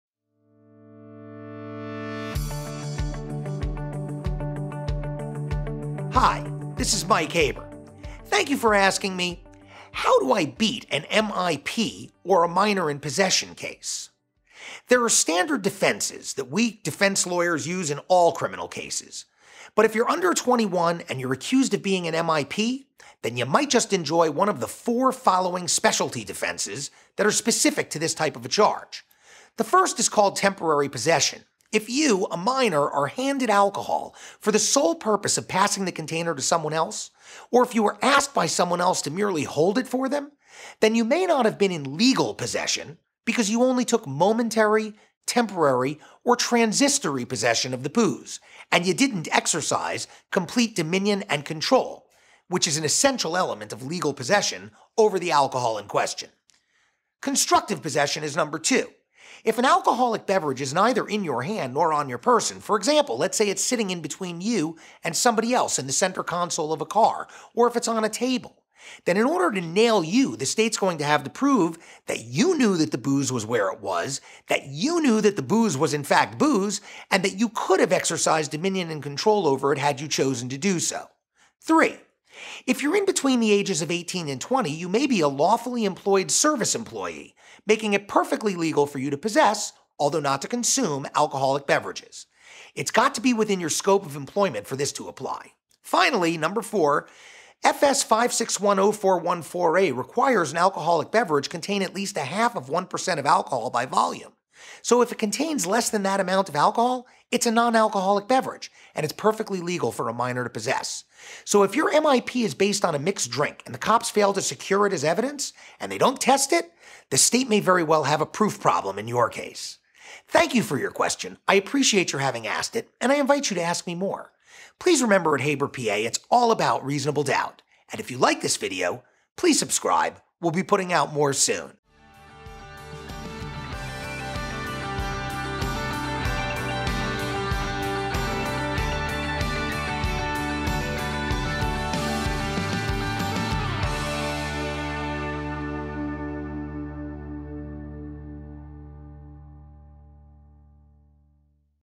225) How to Beat a “MIP” / “Minor in Possession” Case? ~ HaberPA Miami Criminal Defense DUI Lawyer
Thank you for asking me “How do I Beat a “MIP” / “Minor in Possession” Case?

There are standard defenses that we use in all criminal cases, but if you’re under 21 and are accused of being a MIP (Minor in Possession) then, you might just enjoy one of the 4 following defenses that are specific to your charge:

1) Temporary Possession: If you, a minor, are handed alcohol for the sole purpose of passing the container to someone else, or if you were asked by someone else to merely hold it for them, then you may not have been in legal possession because you only took momentary, temporary, or transitory possession of the booze, and you didn’t exercise complete “dominion and control” - which is an essential element of legal possession - over the alcohol.

2) Constructive Possession: If an alcoholic beverage is neither in your hand nor on your person (for example it’s sitting in between you and someone else, say in a center console of a car or on a table) then to nail you the State will have to prove 3 things:  A) that you knew that the booze was where it was, B) that you knew that the booze was booze and C) that you could have exercised dominion and control over it.

3) If you’re in between 18 & 20 then you may be a “Lawfully Employed Service Employee”, making it perfectly legal for you to possess (but not to consume) an alcoholic beverage, provided that it’s within the scope of your employment.

4)  F.S. 561.01(4)(a) requires that a an alcoholic beverage contain at least 1/2 of 1% alcohol by total volume, so if the beverage contains less than this amount then the beverage is “non-alcoholic” and is legal for a minor to possess.  So if your MIP is based on a mixed-drink and the cops fail to secure the mixed drink as evidence then the State may very well have a proof problem.